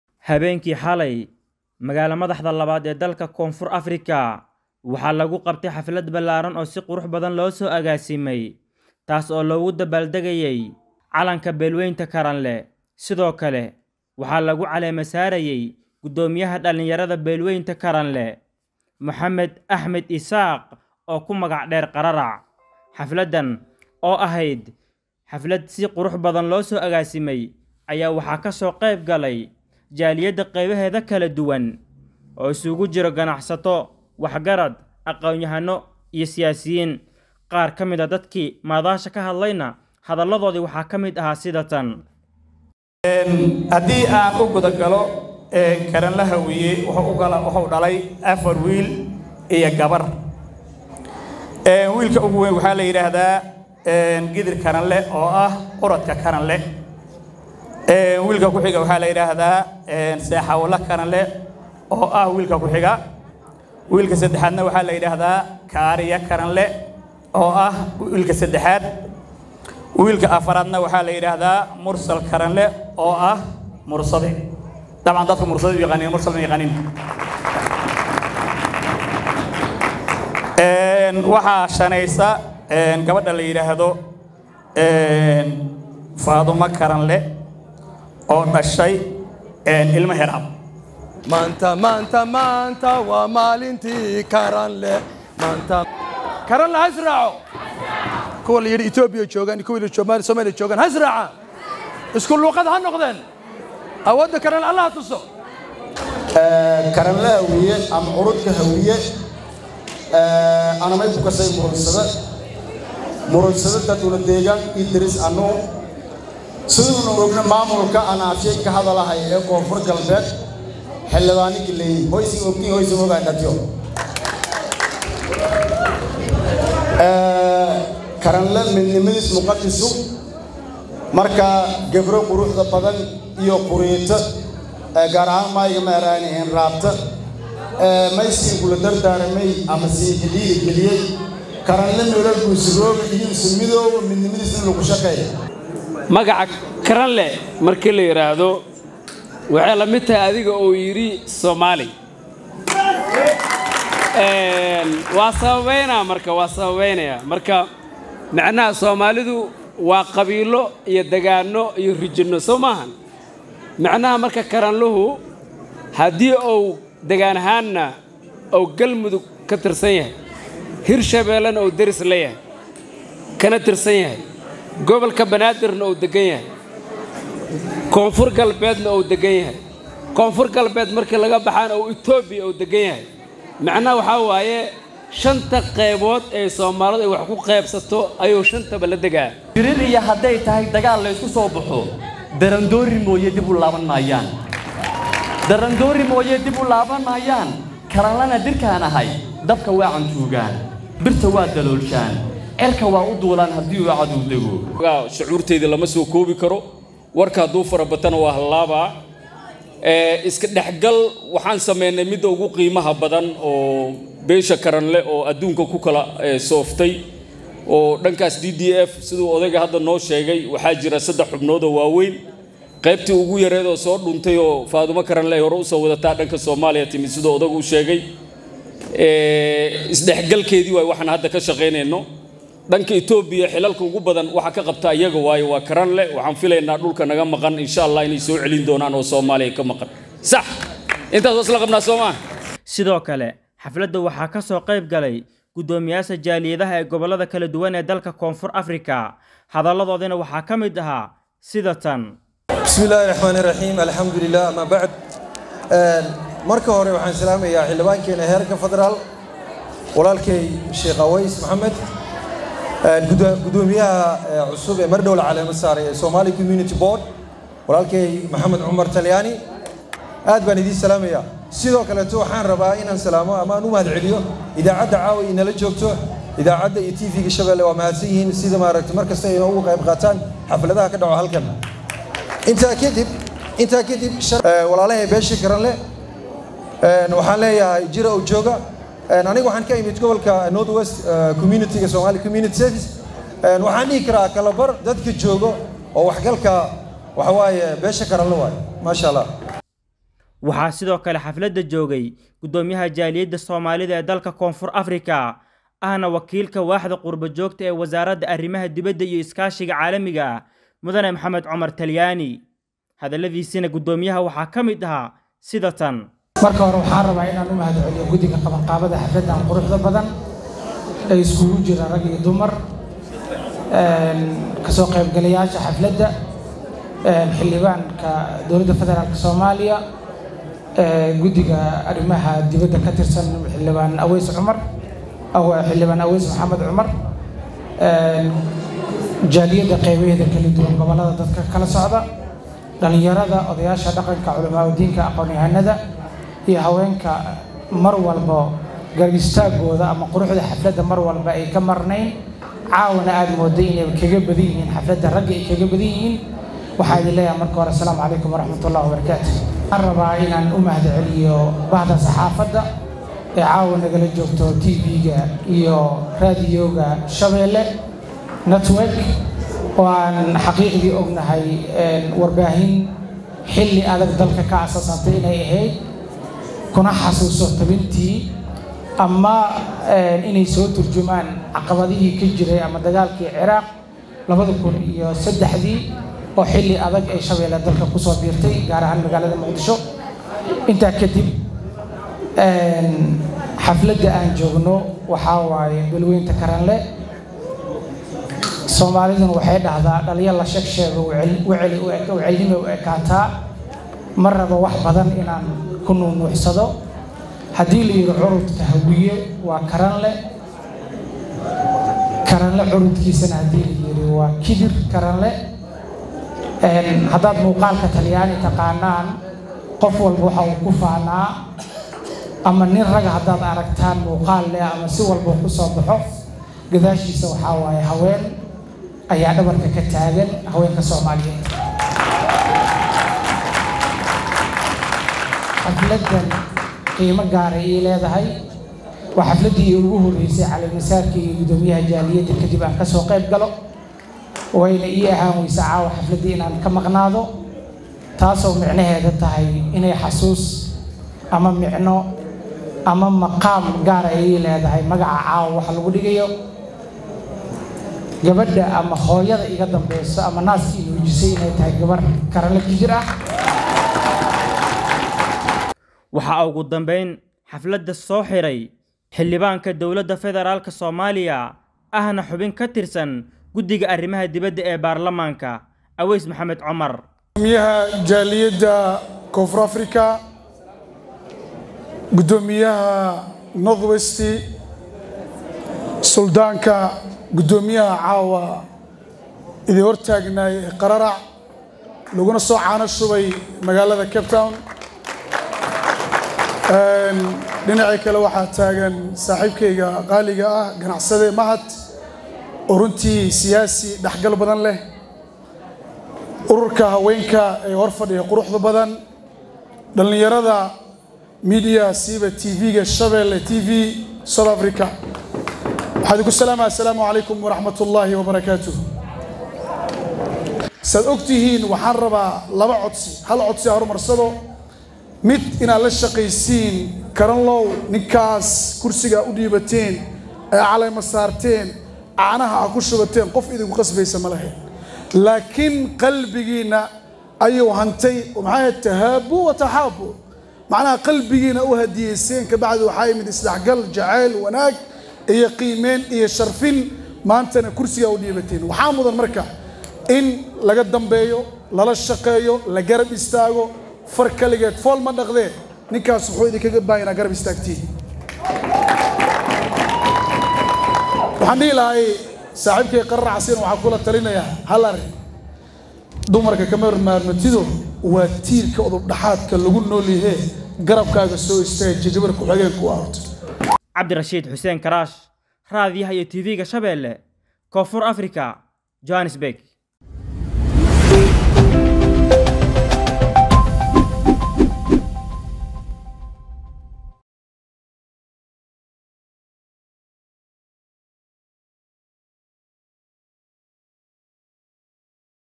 Beelweynta Karanle oo ka mid ah beelaha Soomaaliyeed ayaa magaalada Johannesburg ku qabsatay........
Beelweynta Karanle oo ka mid ah beelaha Soomaaliyeed ayaa magaalada Johannesburg ku qabsatay xaflad ay ugu dabaal dageyso Calankeeda.